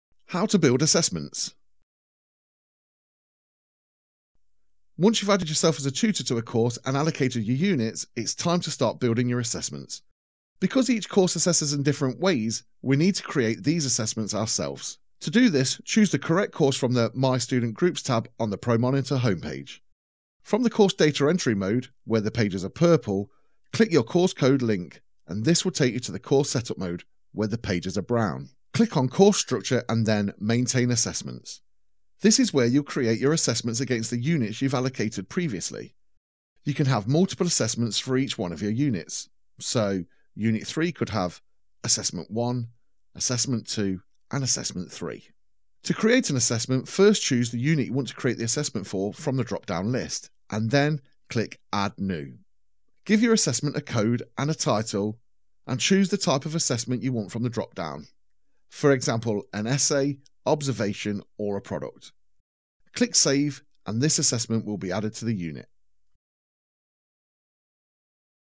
Step 03 ProMonitor Markbook Tutorial - How to Build Assessments
You've added yourself to a course and allocated units, now you need to build assessments